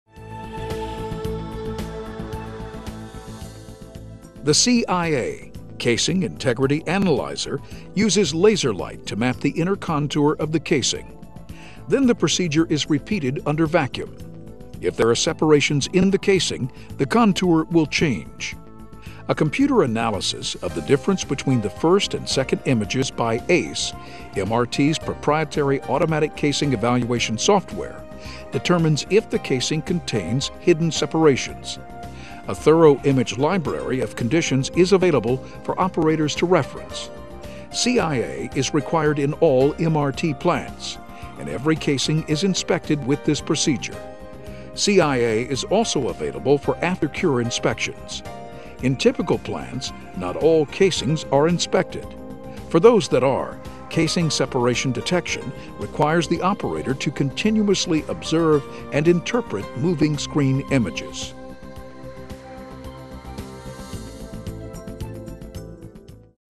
MICHELIN CIA
Analizador de integridad de los cascos (Casing Integrity Analyzer®, CIA)

En cada casco, el sistema CIA utiliza rayos láser para delinear el contorno interno del casco. Luego se repite el procedimiento en condiciones de vacío. Si existen desprendimientos en el casco, el contorno cambia. Con el software de evaluación automatizada de cascos (Automated Casing Evaluation, ACE), se analiza la diferencia entre la primera imagen y la segunda, y se determina si el casco presenta desprendimientos en el cinturón.